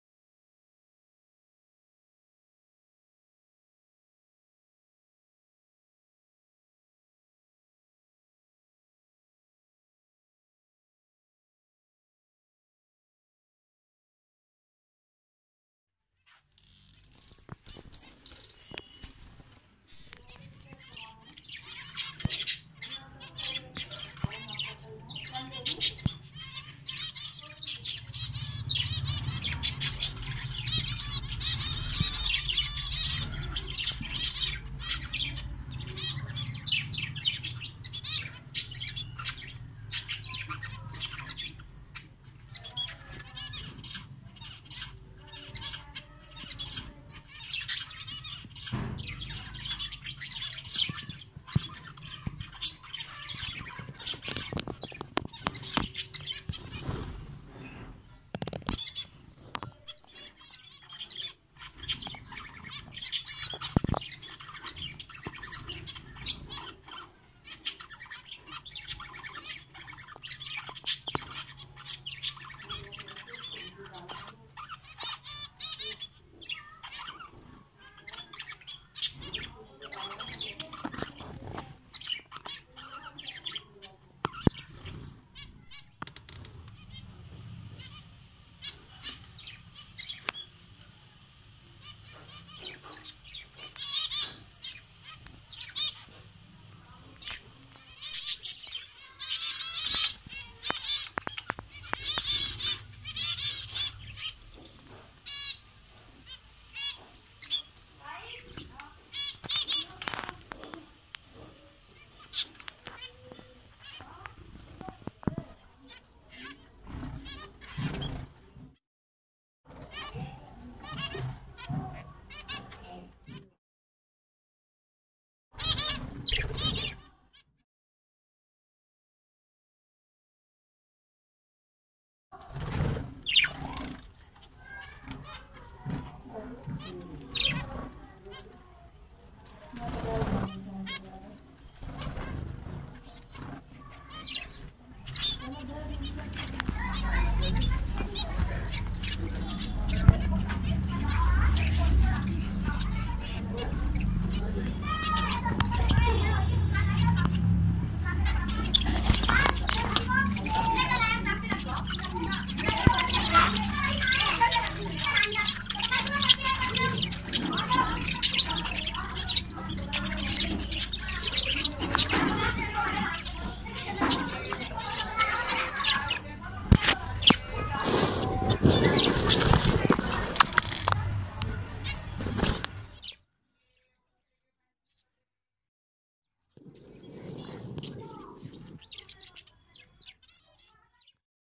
Cute Finch Birds Making A Beautiful Nest | YO YO Amrohi Record
Hello Dosto Kaise Hain Aap Sab ..
Dosto Is Video Mai Humne Apne Finch Birds Ko Apna Nest Banane Ki Video Banai Hai Dosto Video Dekhein Or Enjoy Karein.